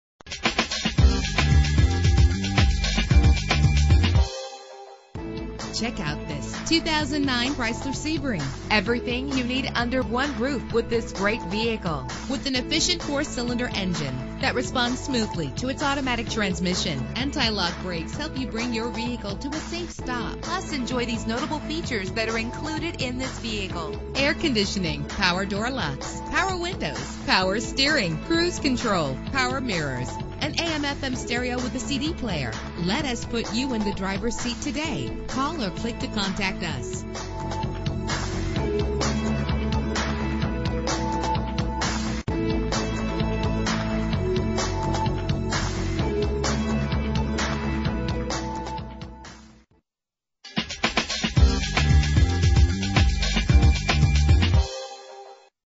2009 Chrysler Sebring - Pittsburgh PA
Year: 2009
Make: Chrysler
Model: Sebring
Trim: LX
Engine: 2.4 liter inline 4 cylinder 16 valve
Transmission: 4-Speed Automatic
Color: Light Sandstone Clearcoat Metallic
Mileage: 50
Address:
2690 West Liberty Ave